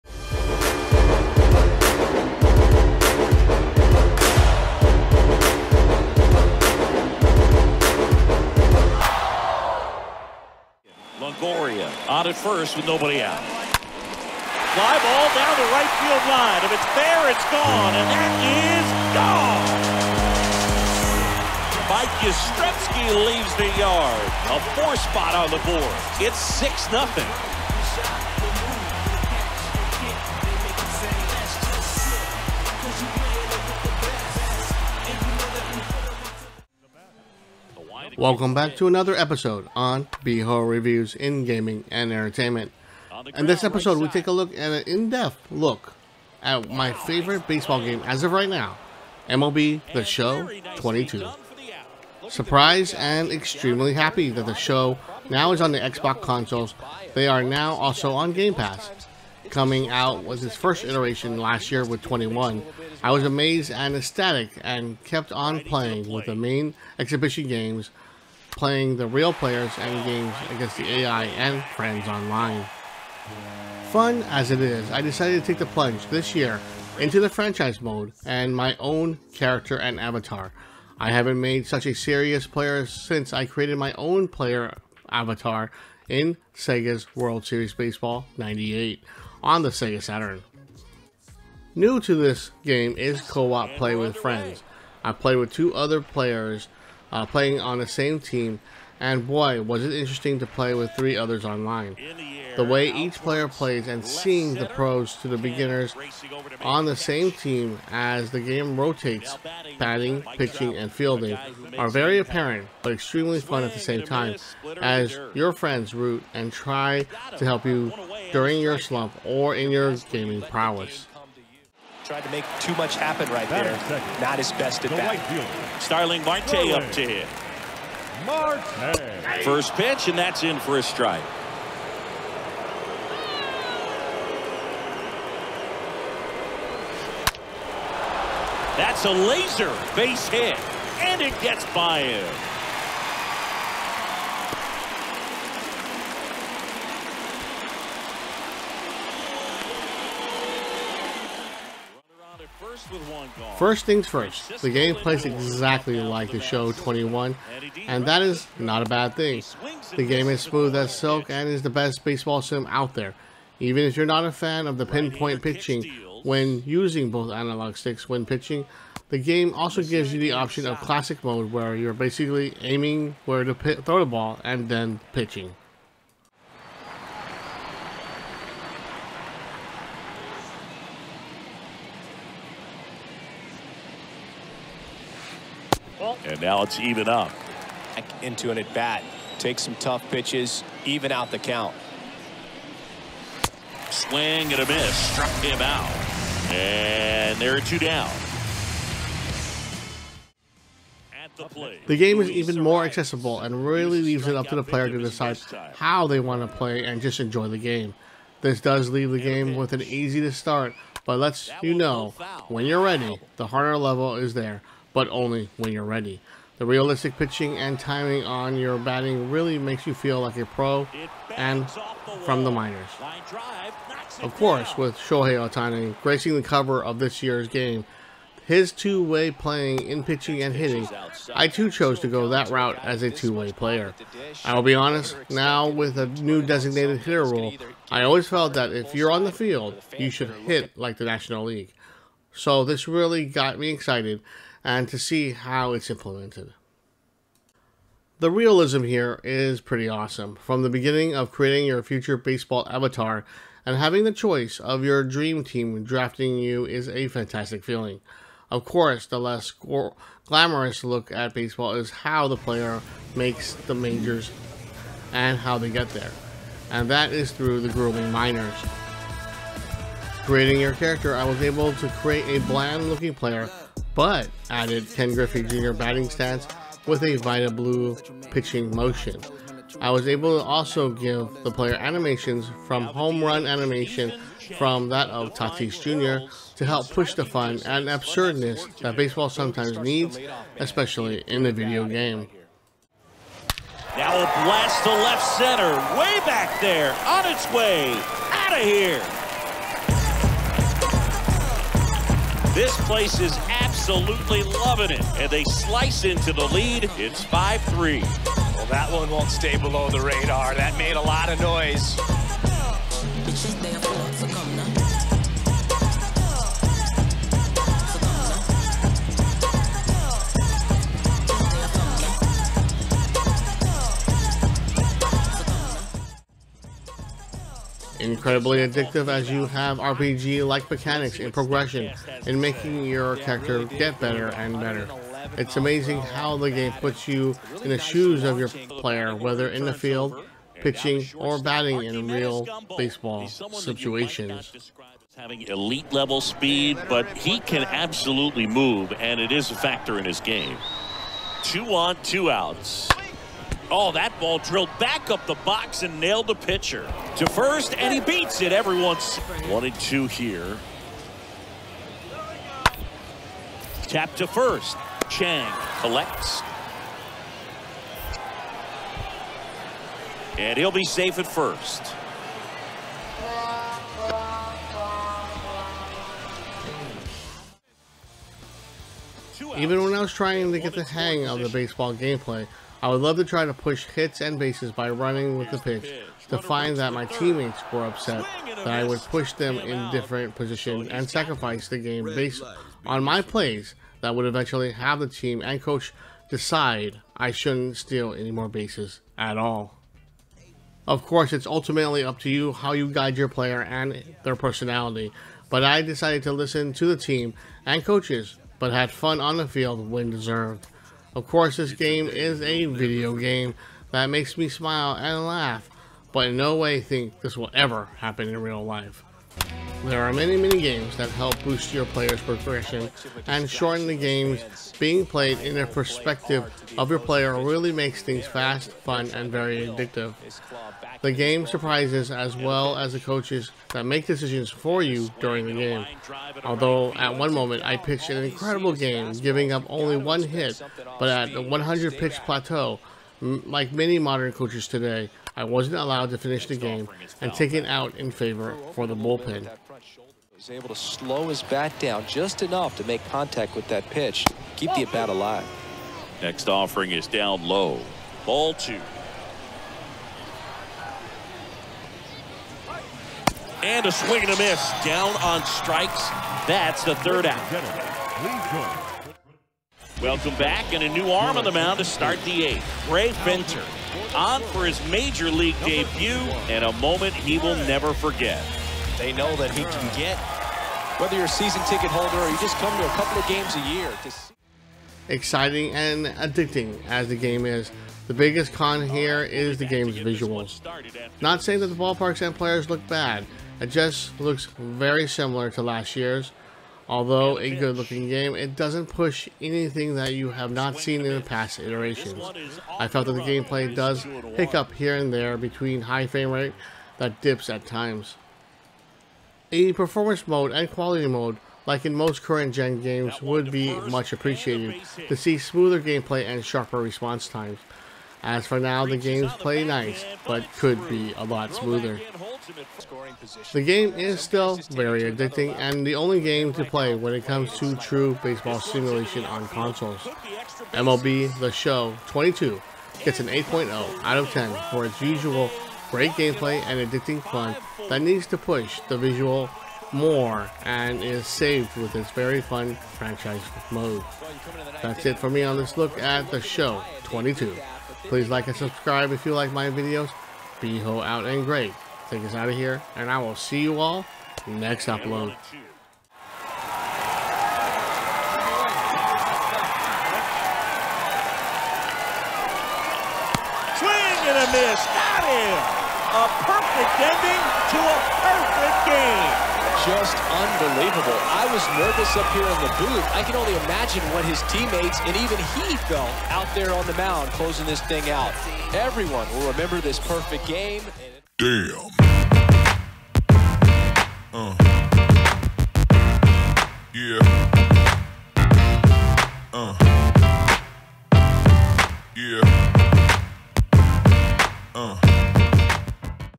MLB THE SHOW 22 Review
The Best Baseball Sim is back with Shohei Ohtani in this Game Review of MLB The Show 22.  MLB The Show 22 is a baseball video game by San Diego Studio and published by Sony Interactive Entertainment, based on Major League Baseball. It was released for the PlayStation 4 PlayStation 5, Xbox One, and Xbox Series X/S, as well as Nintendo Switch, a first for the franchise.